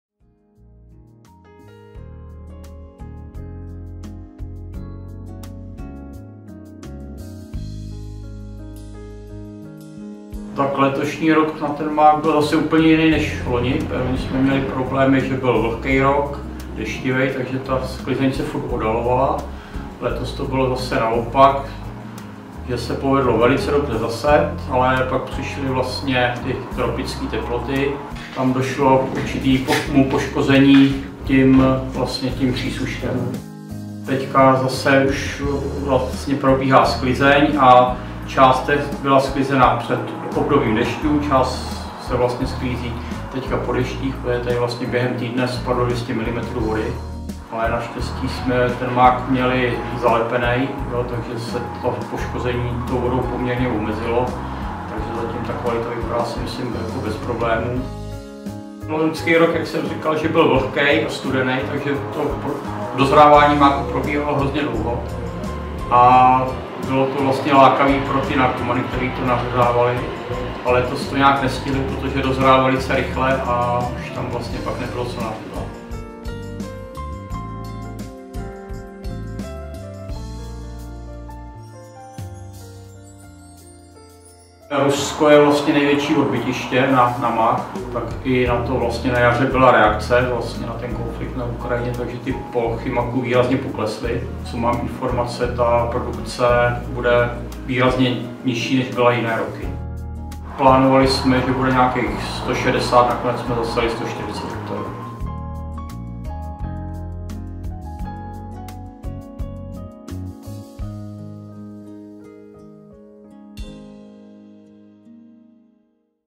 Sklizeň máku 2022, aneb každý rok je jiný | Rok zemědělce
Zemědělci na Vysočině sklidili letošní úrodu máku. Zatímco v době sklizně obilí bylo počasí téměř ideální, s mákem to zkomplikovaly deště. Zrno ale přečkalo v makovicích a podařilo se ho sklidit v dostatečné kvalitě. Letošní rok byl jiný také z pohledu tzv. nařezávačů makovic.